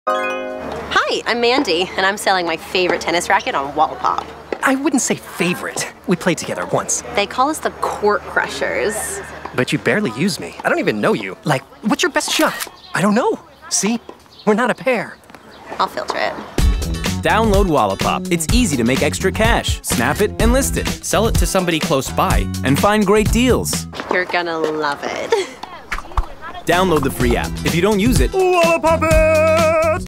Wallapop Commercial - Court Crushers (30 sec)
Mandy tells us about how she's selling her favorite tennis racquet on Wallapop. They have plenty to say to each other about their relationship.  If you don't use it, Wallapop it! Buy and sell nearby with the free app